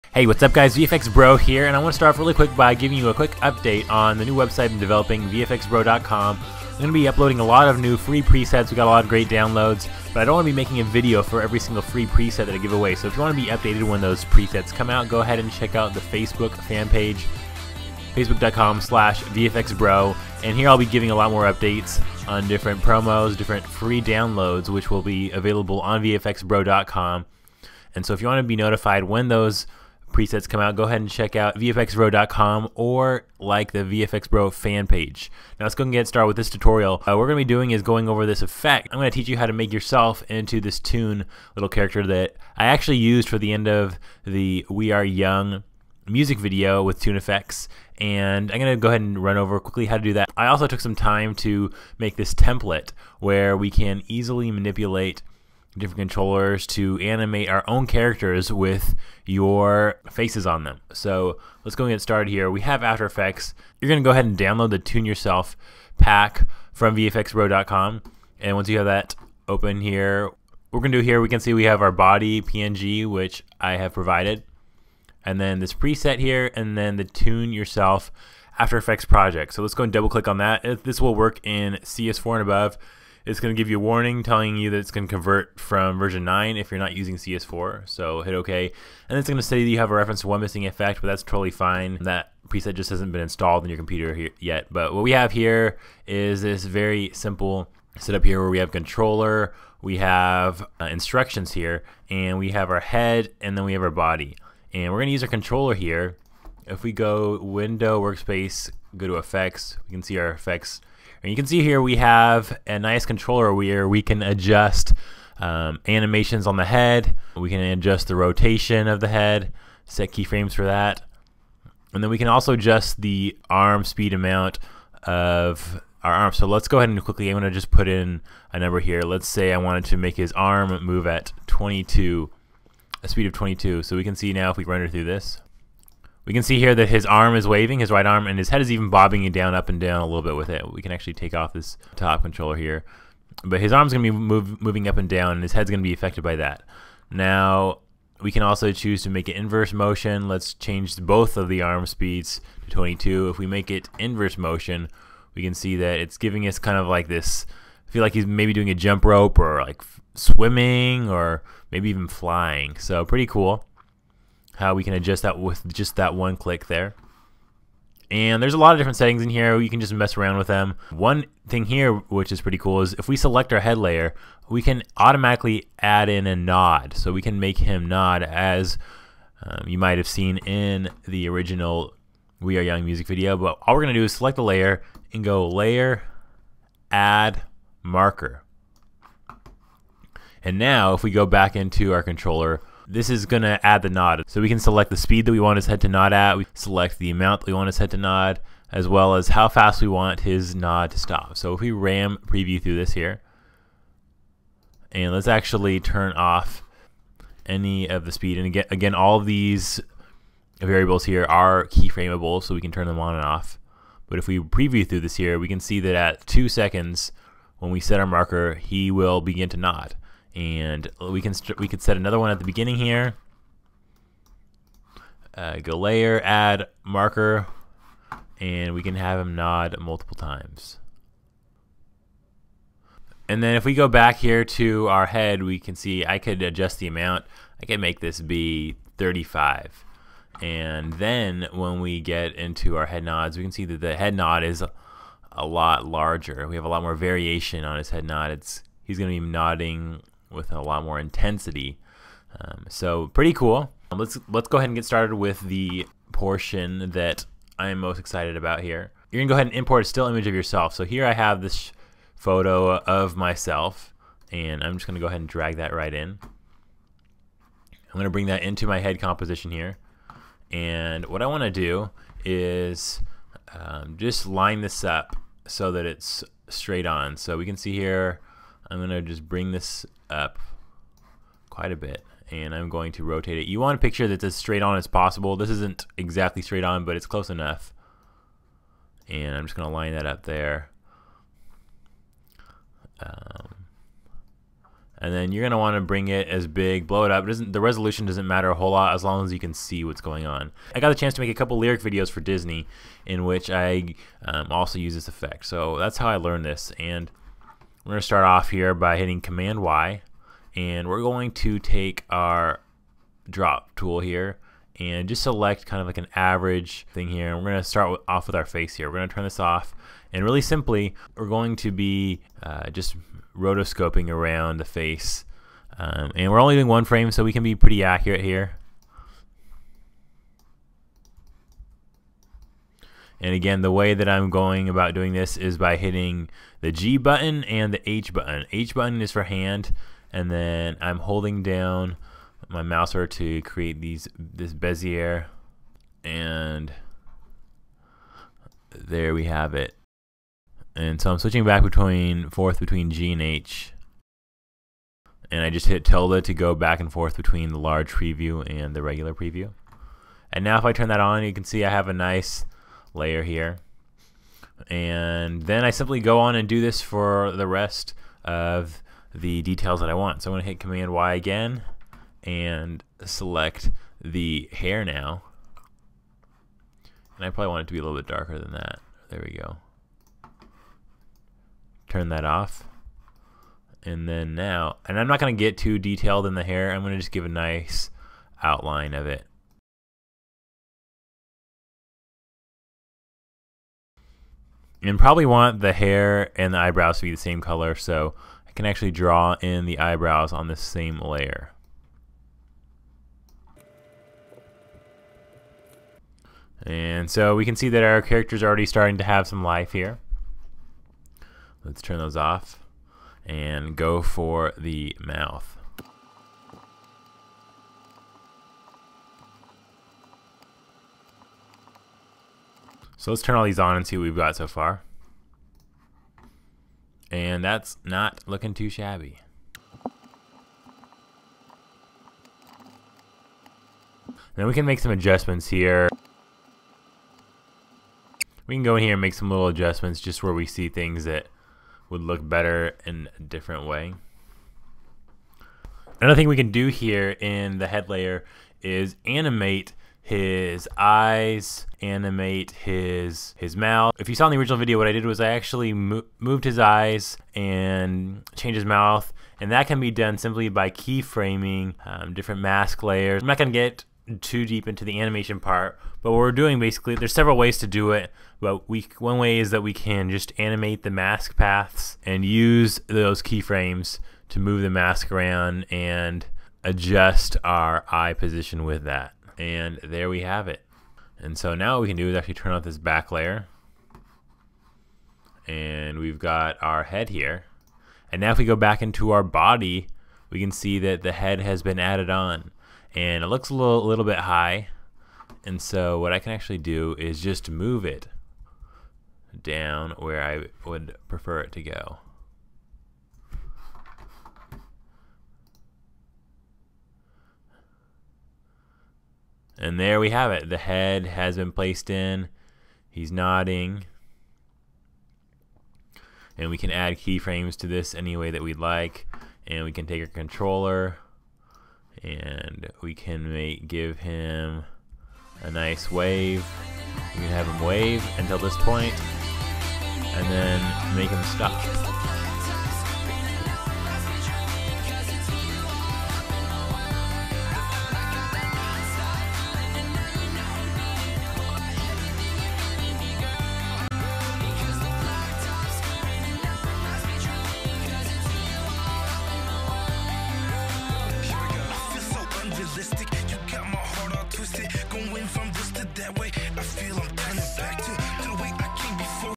After Effects Tutorial Template - ToonYourself
Get Updated on New Free Presets and Templates!

Easily animate yourself as a cartoon using a controller in Adobe After Effects CS4 and above. Enjoy and share the love!

After Effects CS6 Training Coming soon!